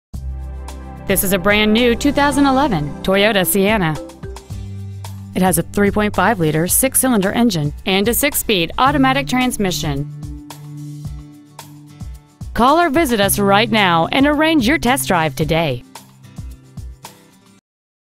2011 Toyota Sienna Denver CO 80221
Year : 2011
 Make : Toyota
 Model : Sienna
 Engine : 3.5L V-6 cyl
 Trans . : 6-Speed Automatic
 Exterior : Silver Sky
 Miles : 6

 201 W. 70th Ave.
 Denver , CO 80221
 2011 Toyota Sienna Denver CO